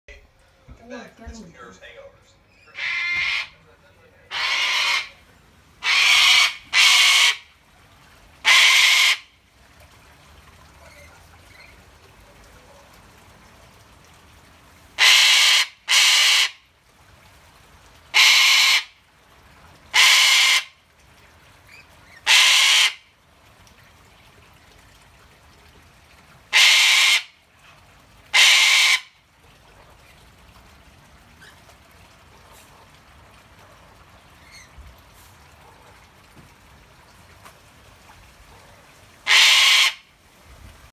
Kookaburra Ignores Noisy Neighbor || ViralHog
Occurred on September 2019 / Sydney, New South Wales, Australia

"An angry cockatoo shouting at a kookaburra that completely ignores him. Must have woken up on the wrong side of the branch."